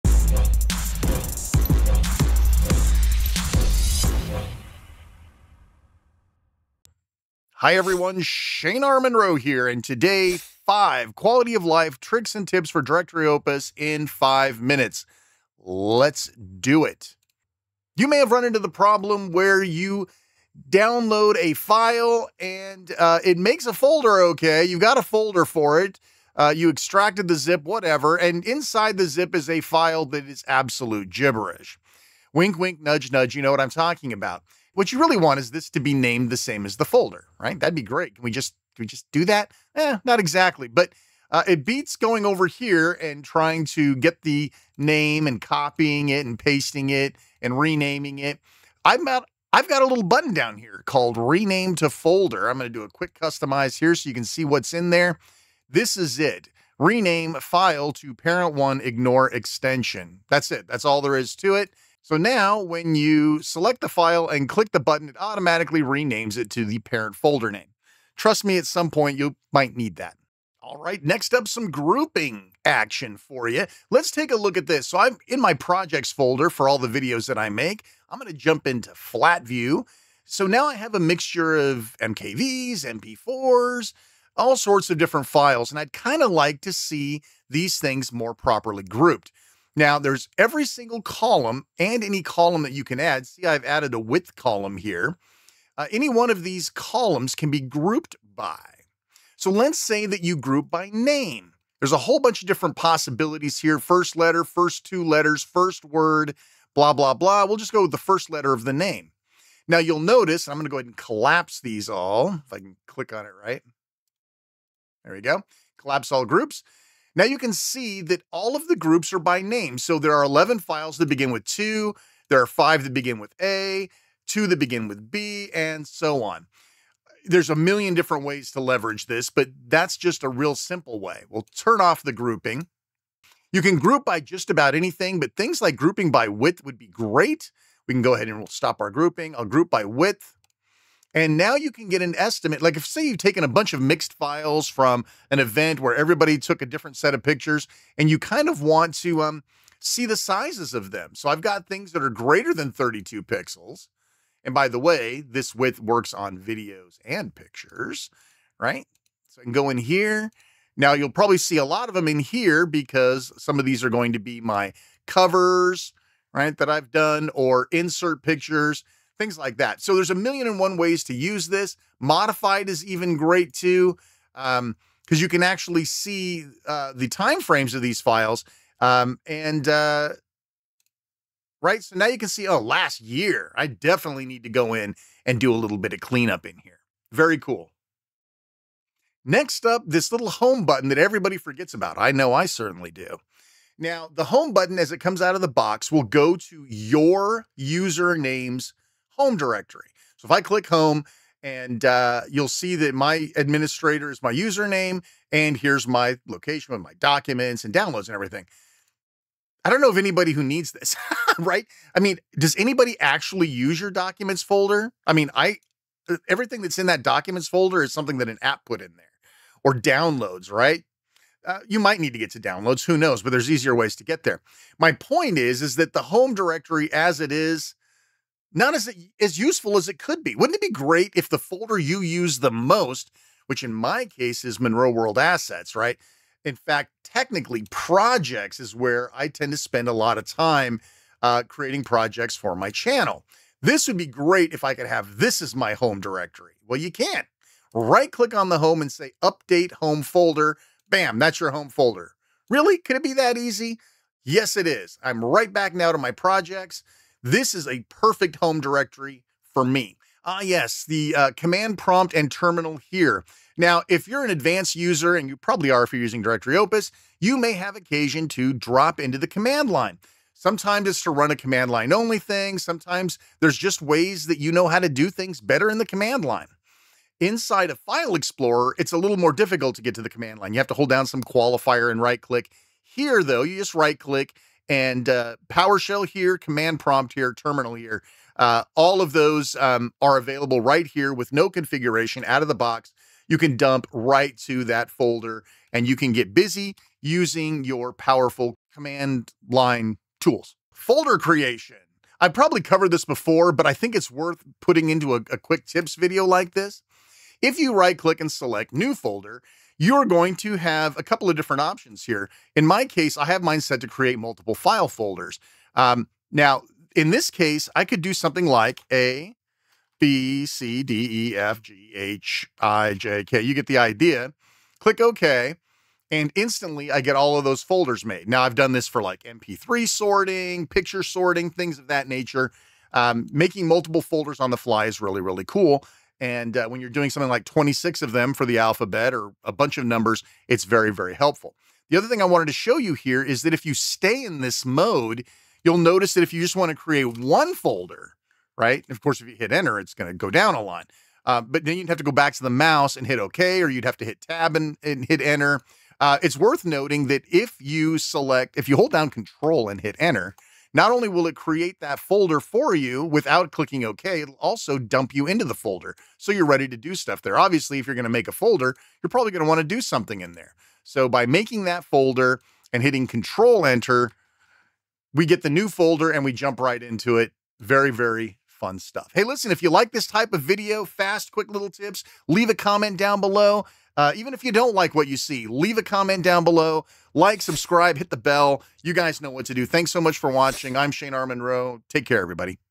Directory Opus 5 in 5 #1 - Tricks, Tips and QOL in Five Minutes
Been awhile since we've had a Dopus video, time to put out a little something.

Here are 5 common tricks I use every single day.  Hope you enjoy it.

Note: I noticed in review that there is an audio thing going on where my voice ducks down a little.  Could be Davinci Resolve 19 beta or the capture method.  I apologize for the inconvenience.